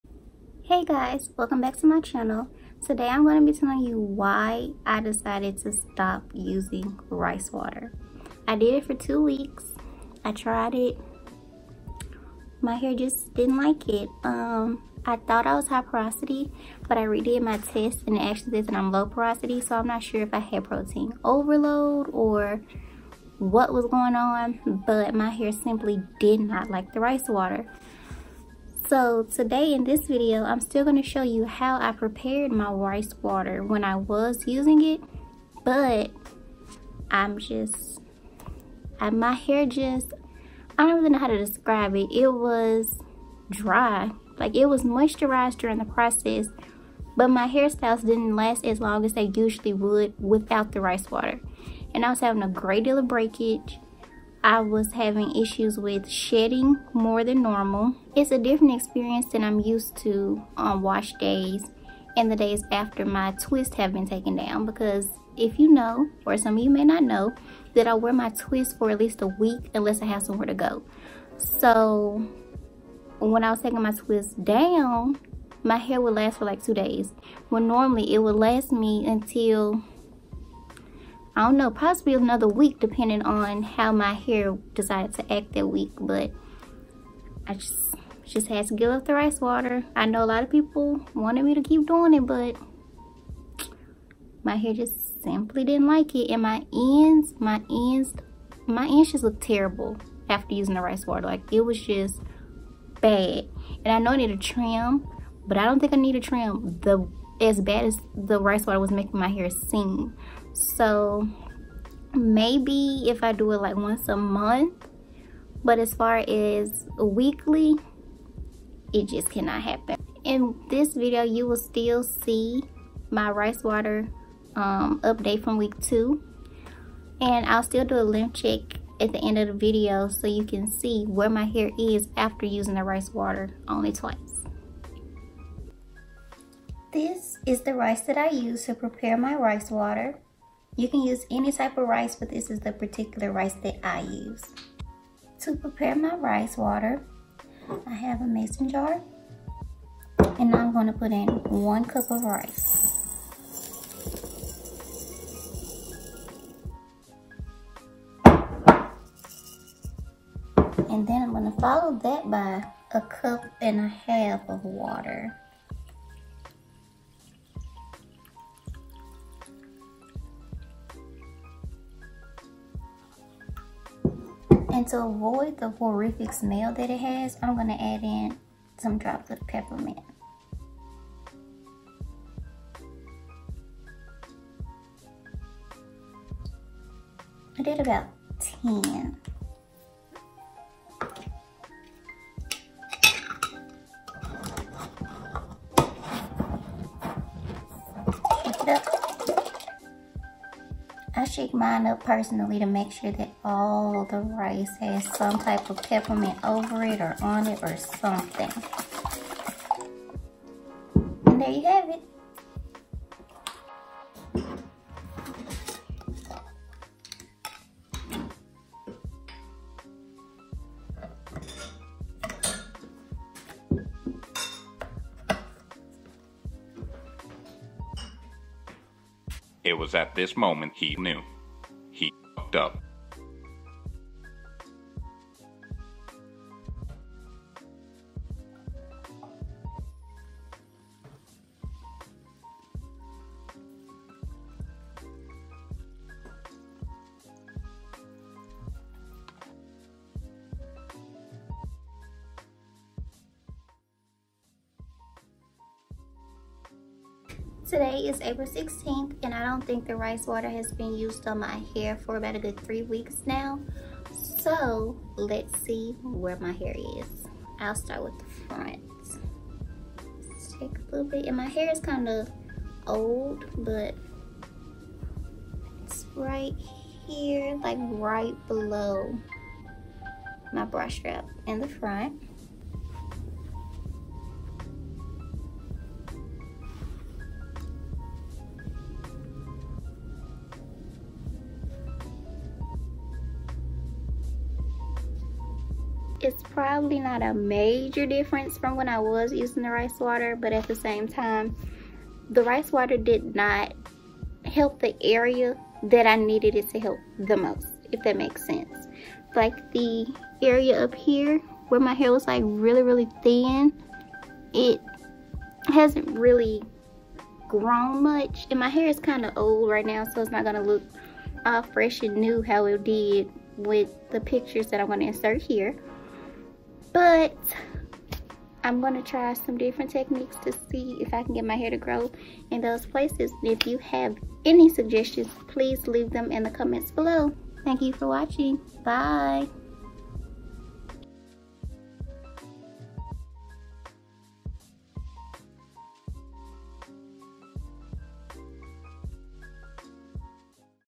DIY 30 Day Rice Water Challange + Demo on High Porosity Hair | NaturallyMarshea
Hi guys! I made the decision to stop using rice water. My hair just did not like it at all. It became brittle and merely a mess. My ends were horrible looking and atrocious.